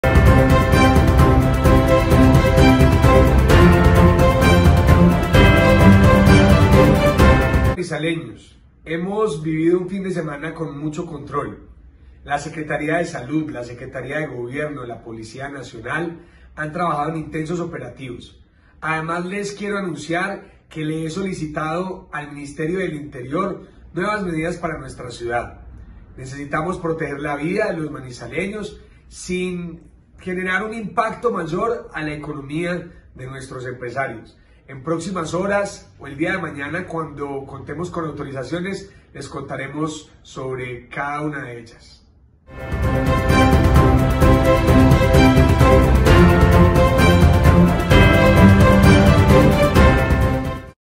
Alcalde de Manizales pidió autorización a MinInterior para tomar nuevas medidas ante la pandemia
Alcalde de Manizales pidió autorización a MinInterior para tomar nuevas medidas ante la pandemia en la capital de Caldas